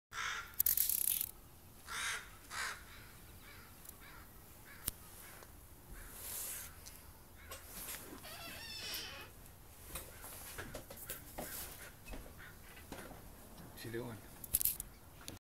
Crow bullies bald eagle
A bald eagle chillin' at the top of a tree in my parents' back yard was harassed out of its spot by a crow.